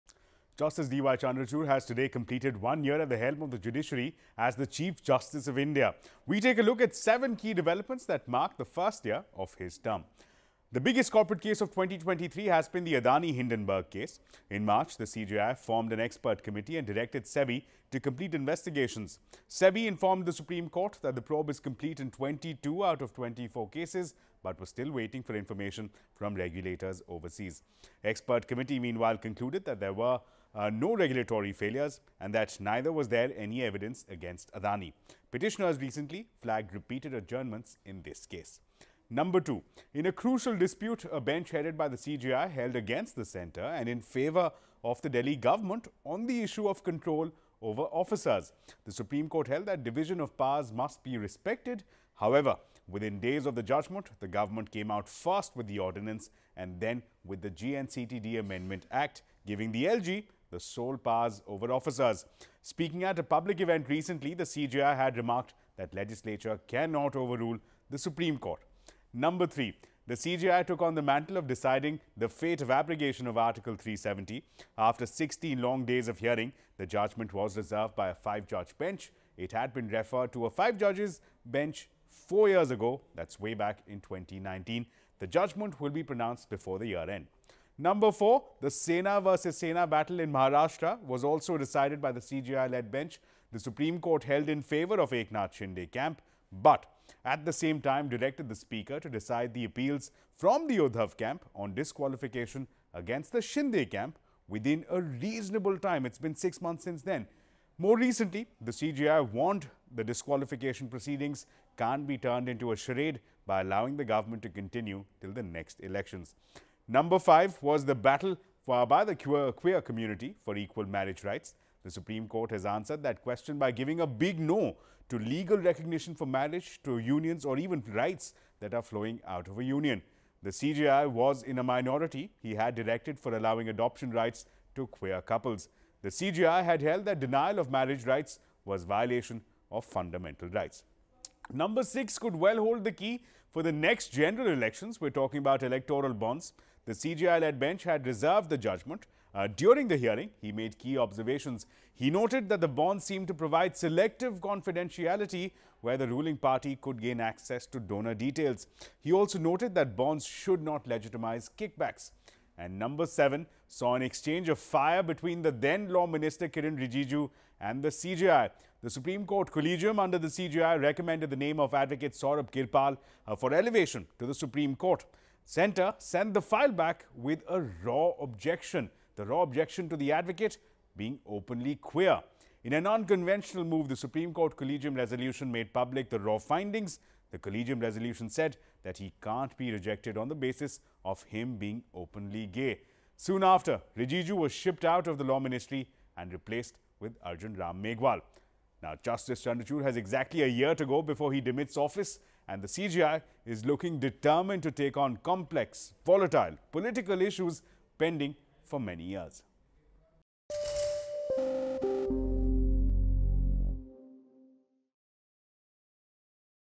A Throwback On CJI DY Chandrachud's 1st Year As CJI | N18V | CNBC TV18
Justice DY Chandrachud has, today, completed 1 year at the helm of the Judiciary as the Chief Justice of India

Ashmit Kumar takes a look back at 7 key developments that mark the first year of his term.

n18oc_business